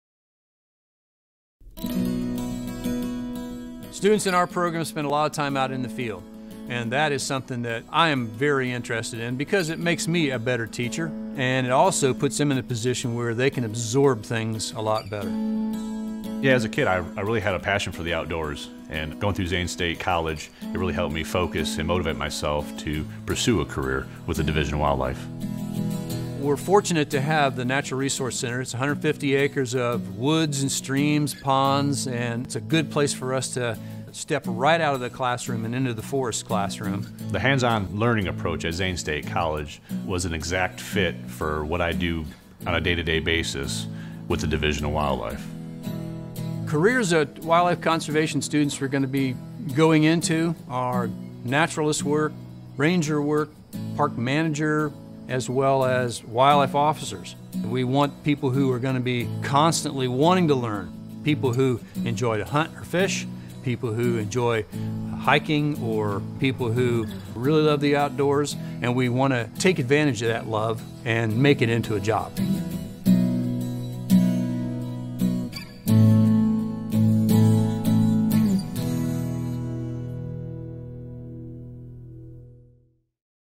Wildlife Conservation
Picture yourself in class. Were you imagining streams, trees, and plenty of wildlife? In this program, your main classroom is the 150-acre Natural Resource Center. You’ll explore a wetland, prairie demonstration areas, wildlife enhancement projects, and field and forest experiment sites. Watch to learn more!